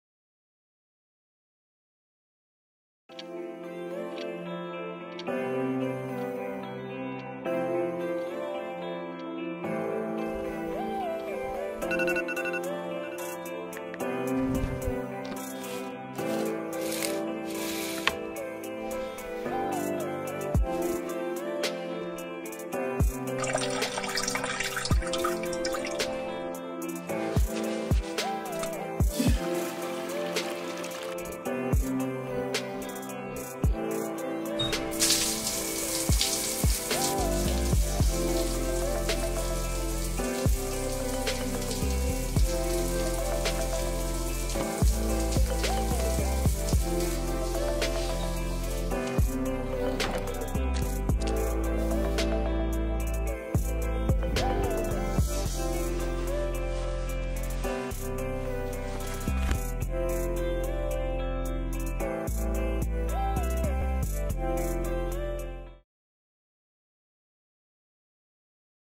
Zhu Yuan's Morning(Zenless Zone Zero Animation)
All the scenes in this video are made by Lanwen Tunzi Studio